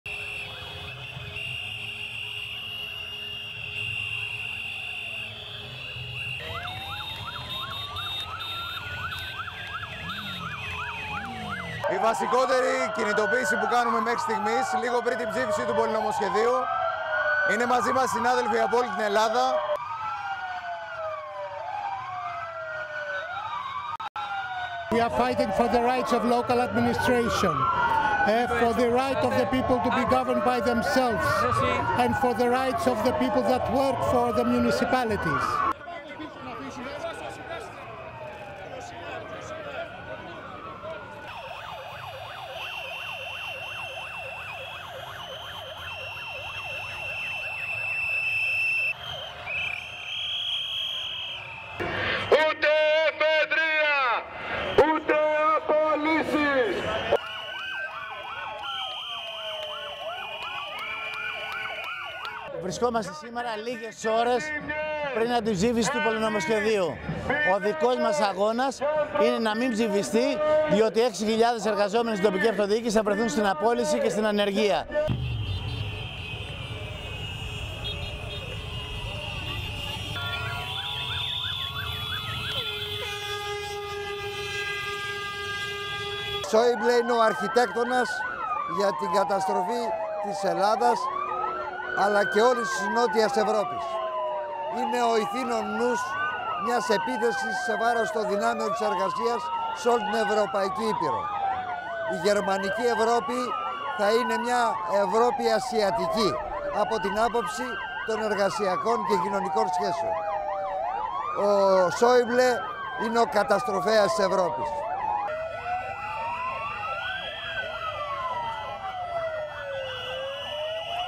Protests ahead of parliament's vote on public-sector job cuts
AP TELEVISION
1. Policewoman in uniform riding a bike, blowing a whistle, and holding up Greek flag
2. Mid of local police woman clapping and blowing whistle as she rides motorbike with a colleague in uniform
3. SOUNDBITE (Greek) Petros Gavrileas, local policeman:
"This is the most important and large scale protest we have staged before the voting of the bill. Colleagues from all over Greece have come here."
4. Police cars lined up, blocking a major avenue in downtown Athens as police officers stage protest
5.SOUNDBITE (English) George Kaminis, Athens Mayor:
"We are fighting for the rights of local administration and for the right of the people to be governed by themselves and for the rights of the people that work for the municipalities."
6. Mayor of Athens George Kaminis holding banner with other protesters reading (Greek)
"No to grabbing our responsibilities, no to employee layoffs, and yes to the transfer of employees and overhaul of local government."
7. Local policemen blowing their whistles while riding in a car
8. Municipality workers marching to parliament holding banner reading (Greek) "Get out and take the bailout with you!"
9. Protester with whistle
10. SOUNDBITE (Greek) Themis Valasopoulos, head of municipality employee union:
"We are here just a few hours before the vote in parliament. We are fighting the bill because, if it passes, 6,000 local government workers are about to be laid off."
11.Wide of crowds outside parliament waving flags and holding banners
12. Local policemen and women staging a protest on motorbikes outside parliament
13. SOUNDBITE (Greek) Panayotis Lafazanis, opposition party MP:
"Schaeuble is the architect of Greece's and Southern Europe's catastrophe. He is the mastermind of an attack against workers' rights on the entire Greek continent. German Europe will be an Asian-style Europe when it comes to workers and social rights. Schaeuble has ravaged Europe."
14. Close of large Greek flag spread out on a local police car
STORYLINE
Two thousand local policemen and women staged a protest in downtown Athens ahead of voting on a controversial bill that allows the laying off of public sector workers.
Lawmakers are to vote late on Wednesday on a bill that will put 12,500 public sector staff, mostly teachers and municipal workers,
in a programme that subjects them to involuntary transfers and possible dismissals.
The vote will also pave the way for 15,000 layoffs by the end of next year.
Local policeman Petros Gavrileas said Wednesday's demonstration was "the most important and large scale protest we have staged before the voting of the bill. Colleagues from all over Greece have come here."
The police protesters were joined by school security guards and other local government employees who fear their jobs will be slashed too.
The job cuts were demanded by Greece's emergency creditors, the International Monetary Fund and other countries using the euro.
Staff targeted in the measures took part in a third straight day of protests.
Mayors from around Greece marched to parliament, while municipal police officers rallied through central Athens with their
motorcycles and patrol cars.
"We are fighting for the rights of local administration and for the right of the people to be governed by themselves and for the rights of the people that work for the municipalities," said Athens mayor George Kaminis.